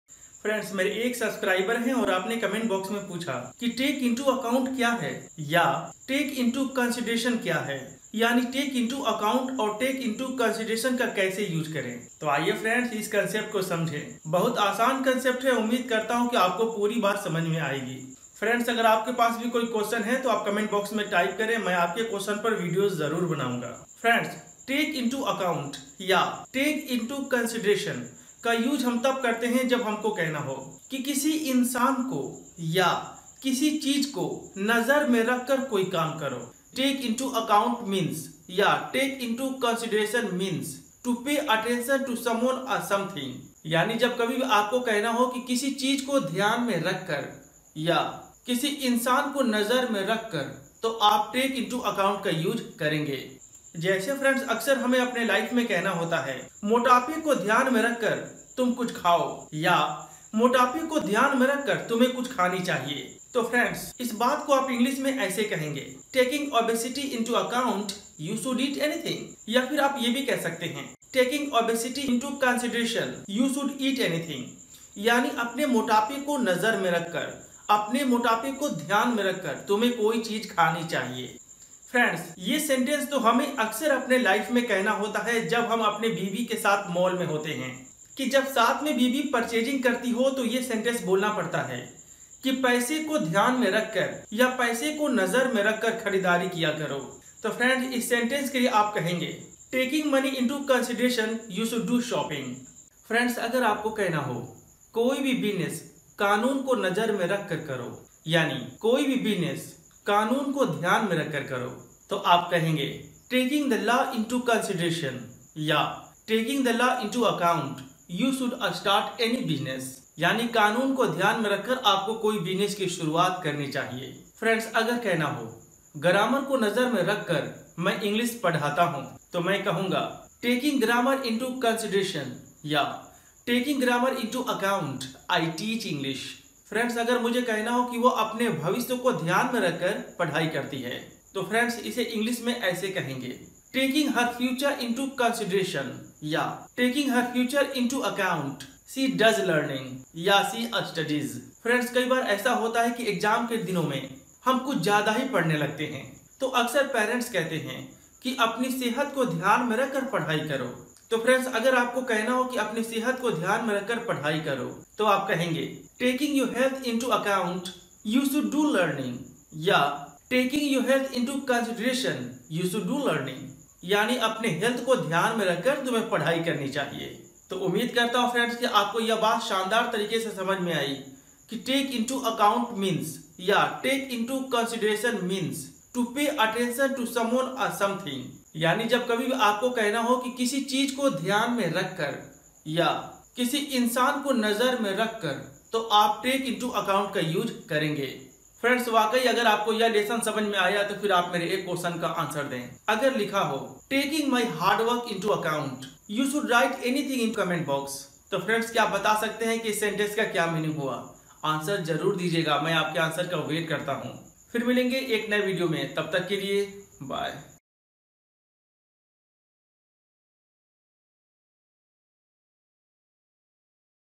Use of Take into account | How to speak Speak English | Learn to speak English | Spoken English.
Hi Friends,
Today I am going to teach you about TAKE INTO ACCOUNT | TAKE INTO CONSIDERATION |